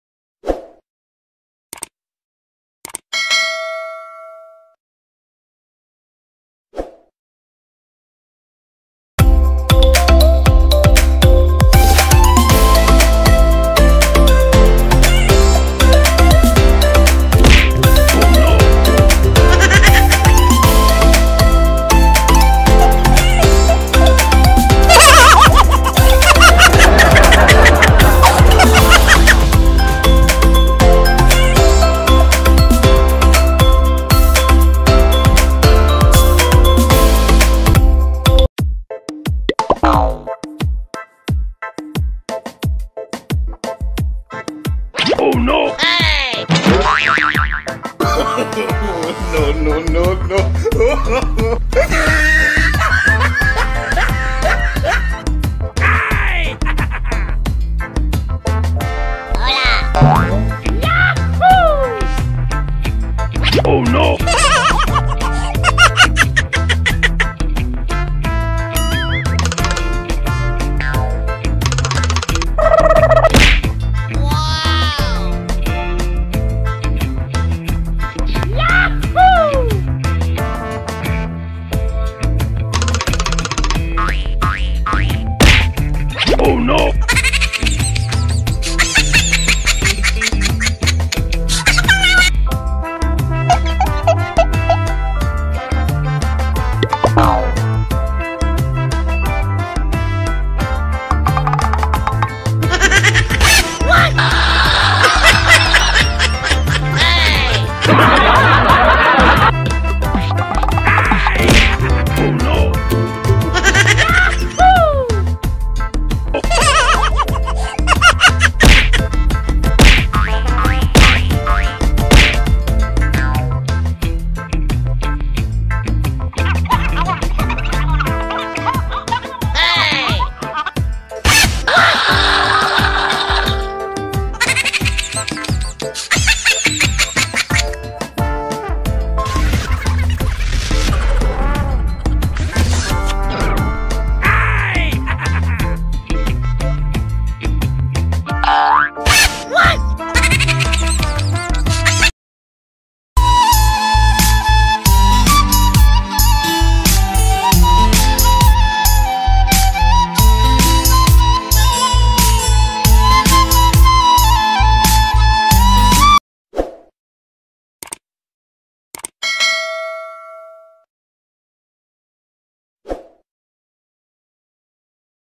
TRY TO NOT LAUGH CHALLENGE || Funny Videos ||New Comedy Video 2019
Must Watch New Funny Video😂😂Top New Comedy Video 2019 |Try To Not Laugh ..
We make this video with some fail funny videos and mixed Some comedy videos. This is village funny videos Our most of the funny videos are making with stupid think And boys stupidity. This video made without any scripting.

This video contains are shooting at that place=Sirajgonj, Bangladesh